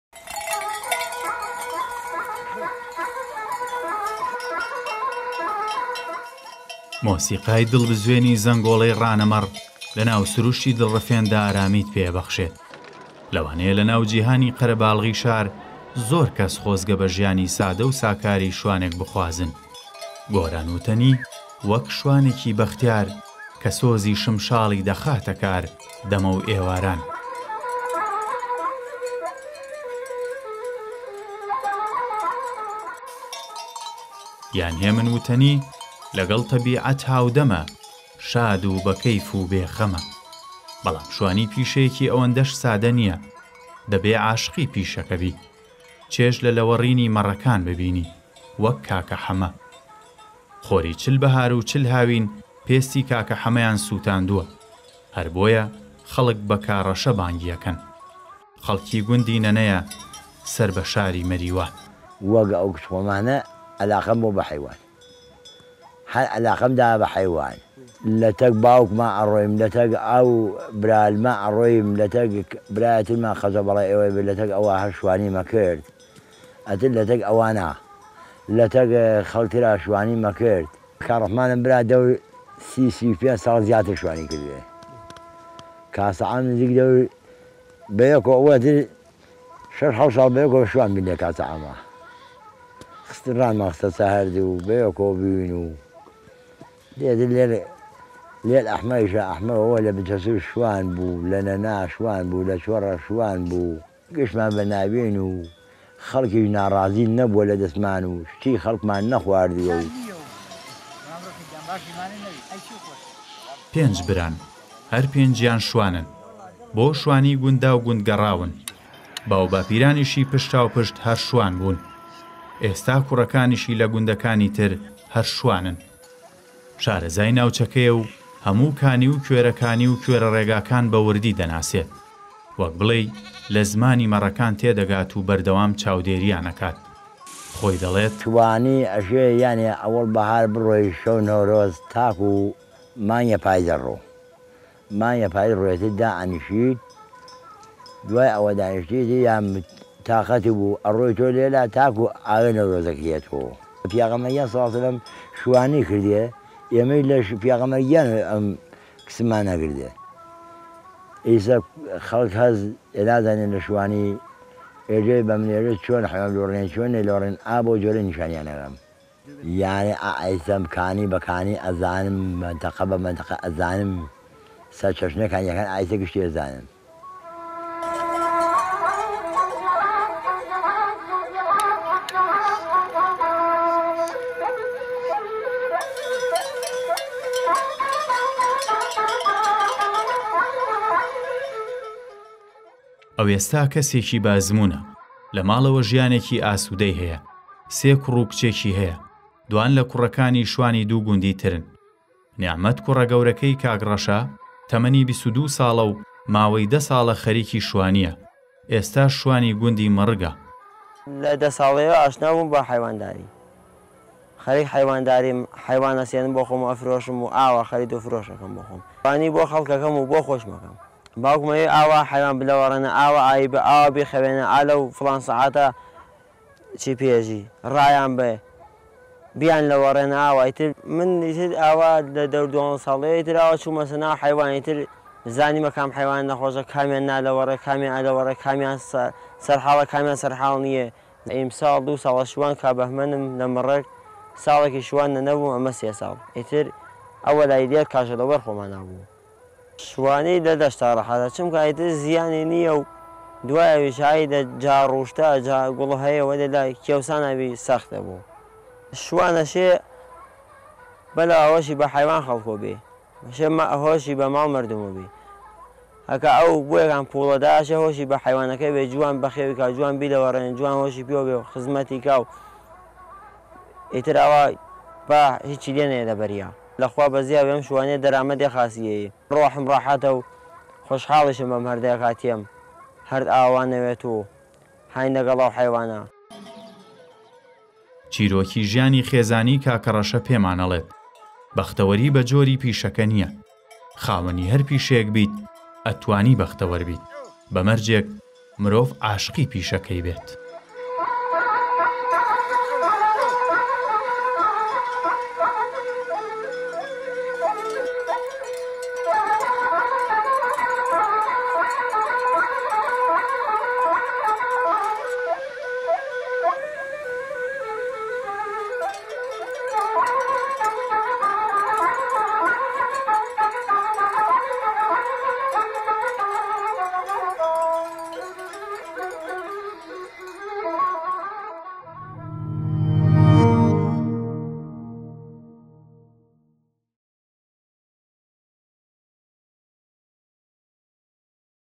شوانێکی بەختیار
شوانێکی بەختیار، بەختەوەری بە جۆری پیشە نییە، هەموو کەسێک دەتوانێت بەختەوەر بێت بە مەرجێک عاشقی پیشەکەت بی